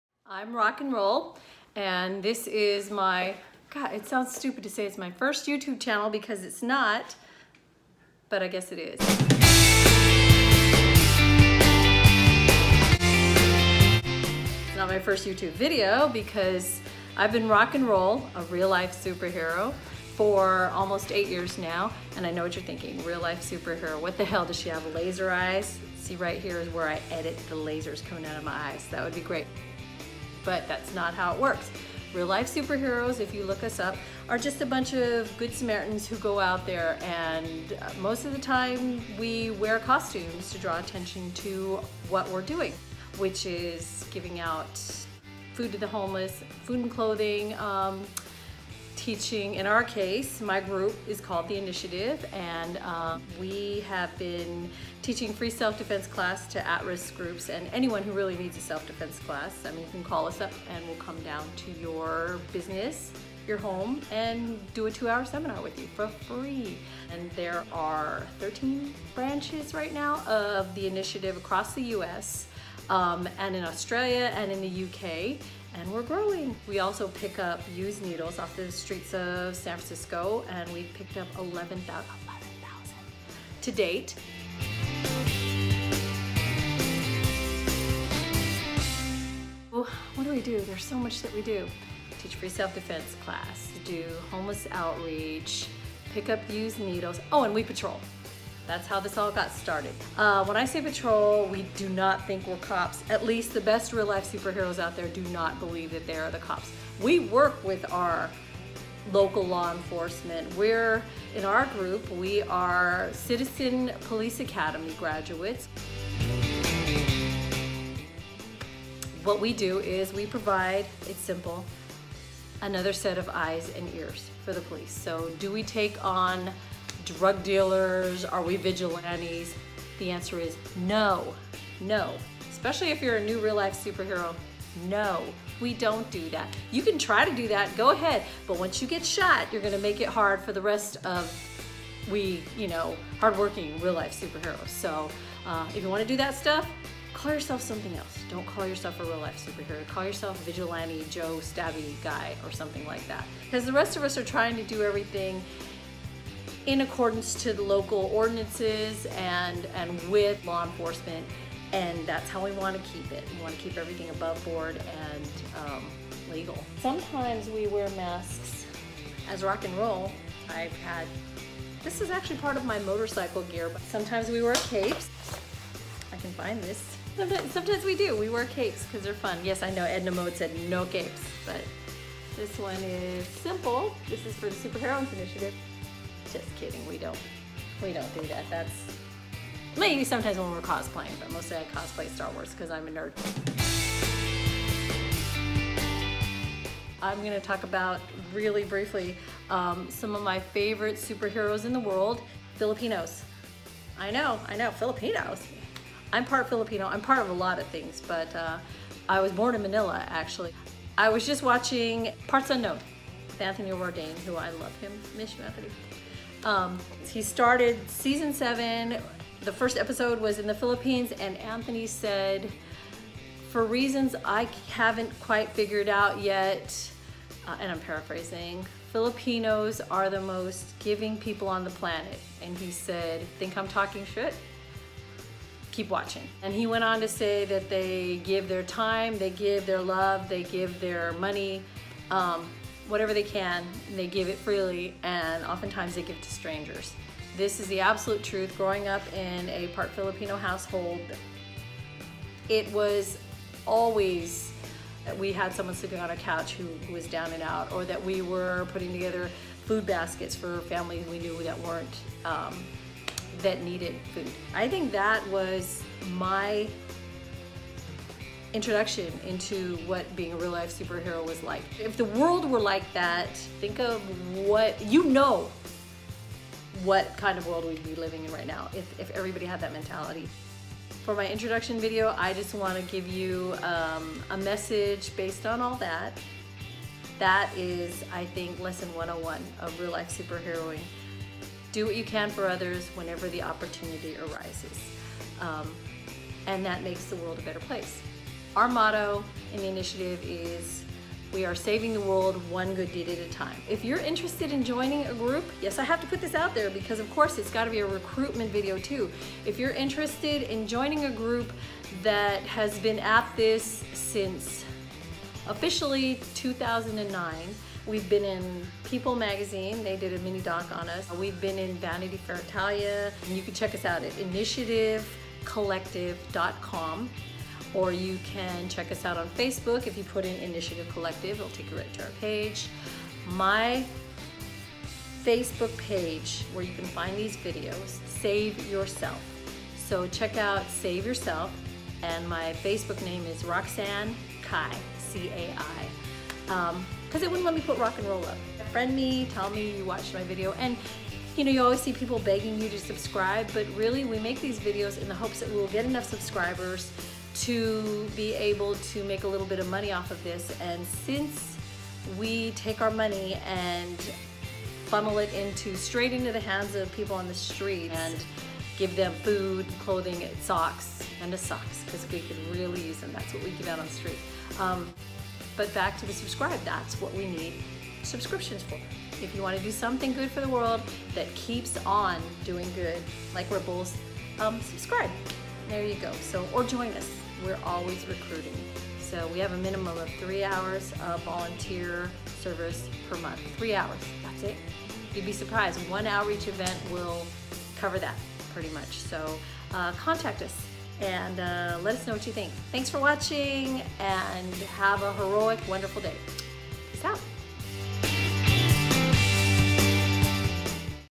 Save Yourself By Rock N Roll
Tips From A Real-life Superhero

Rock N Roll is an author, musician, radio co-host, professional baker, CMT, and real-life superhero.

Founder of the Superheroines Initiative, Co-founder of the California branch of the Initiative, creator and co-host of HEROES 101 Radio, Roxanne Cai - aka "Rock N Roll" in the RLSH world- has taken her experience as a martial arts instructor/CMT/asst. head of security and has been actively "superheroing" in the San Francisco Bay Area and around the U.S. since 2011. Rock has been featured in People Magazine and in a People mini-doc, in Vanity Fair Italia, and various news articles. She appears in the books, Heroes In The Night (Tea Krulos), Mission: Superheroes (Nadia Fezzini), and in the upcoming book, The Hero Gotham Needs (Peter Nowak), and in The Adventures of Miss Fit, a movie about superheroes.

You can learn more about Rock and the real-life superhero group, The Initiative at

She's got tons of videos on her Facebook page. Check them out here: